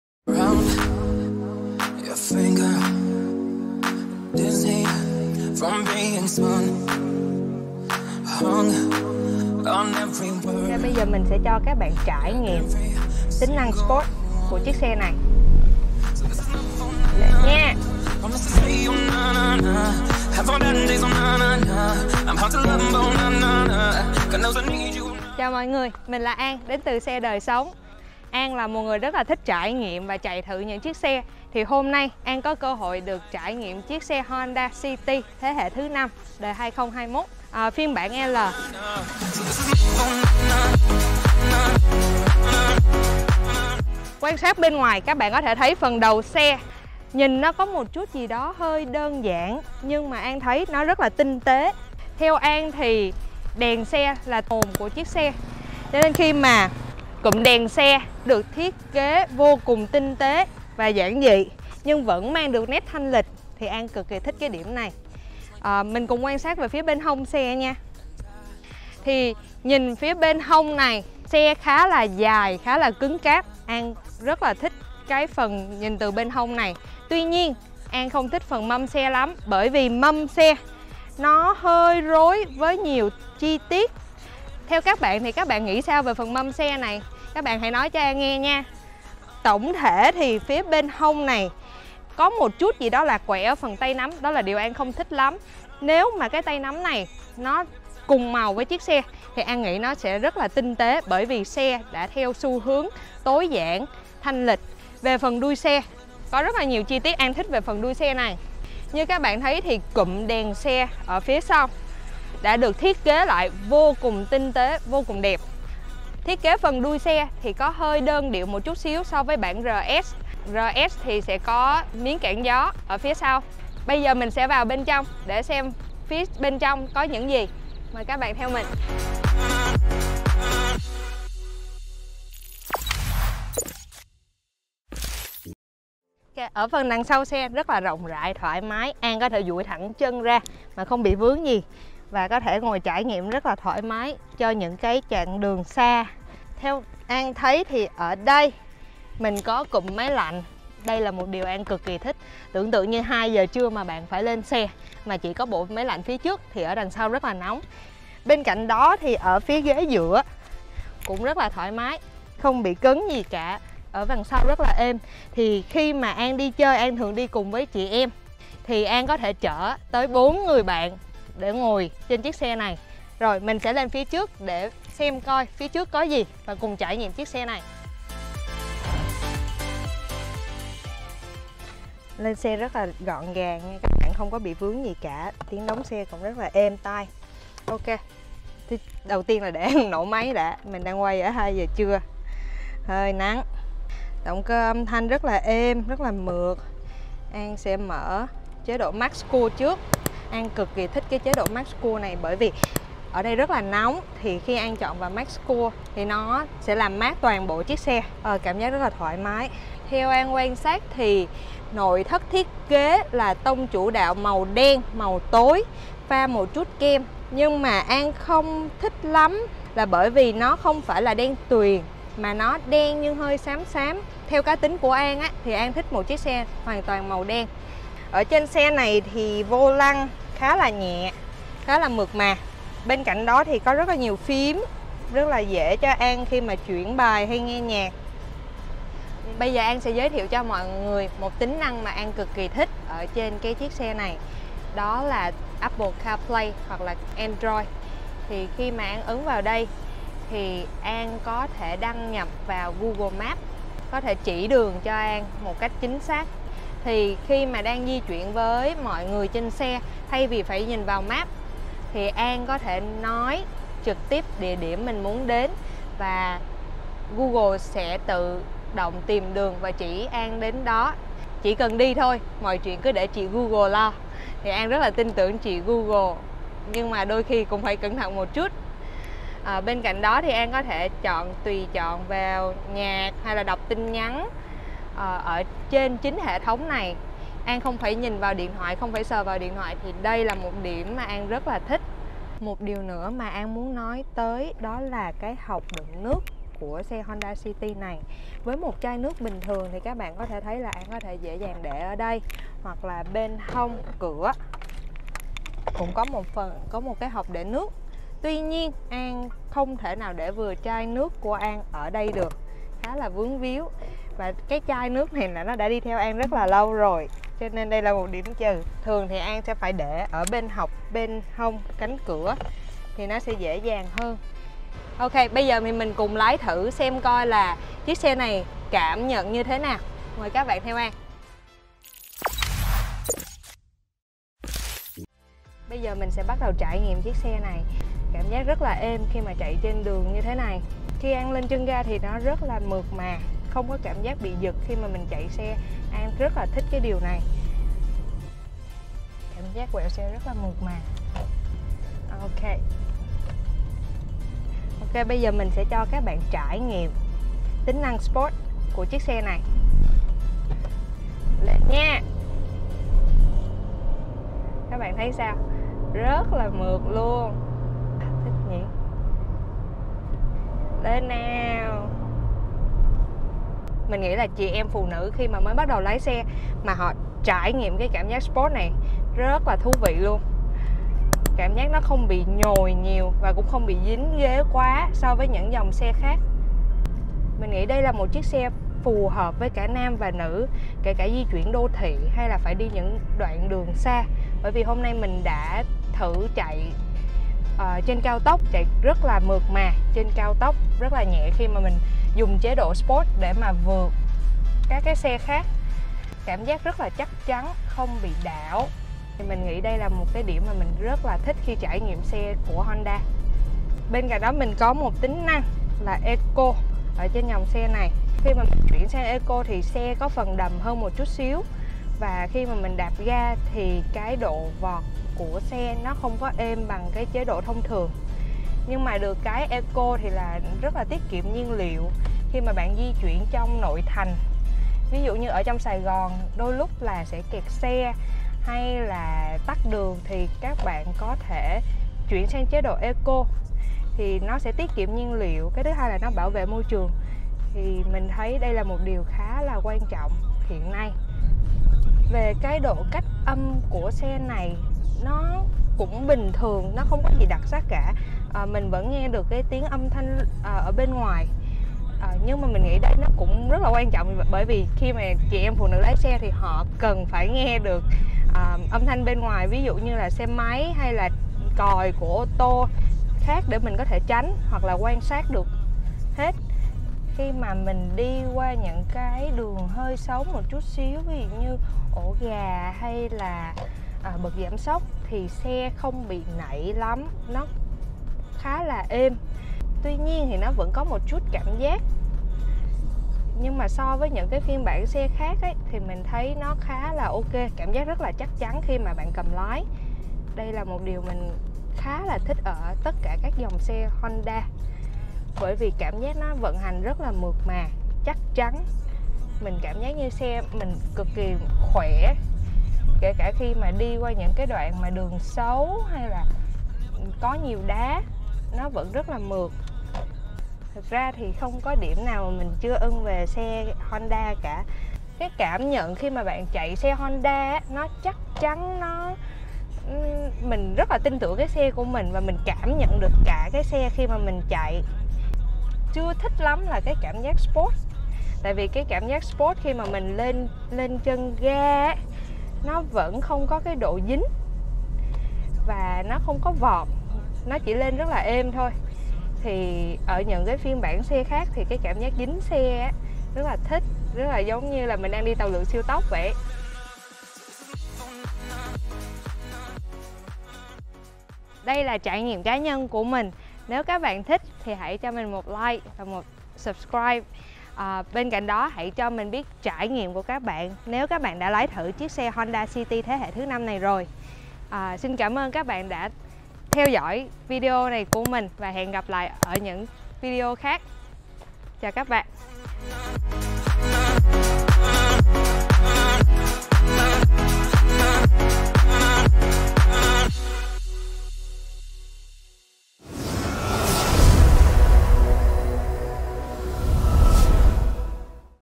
Cùng Ms An Lê trải nghiệm Honda CITY 2021 giá 569tr xem có phù hợp, đáng tiền cho chị em sở hữu?
Cùng Miss An Lê và Xedoisong trải nghiệm chiếc sedan hạng B Honda City 2021 thế hệ mới, phiên bản L (bản giữa) có giá 569 triệu đồng.

- Đánh giá, trải nghiệm Honda City L 2021
- Giá bán Honda City 2021 Việt Nam
- Xedoisong, Honda City 2021
- Chi tiết Honda City L 2021
- Review allnew Honda City 2021

© Bản quyền thuộc về Xedoisong.vn, Việt Nam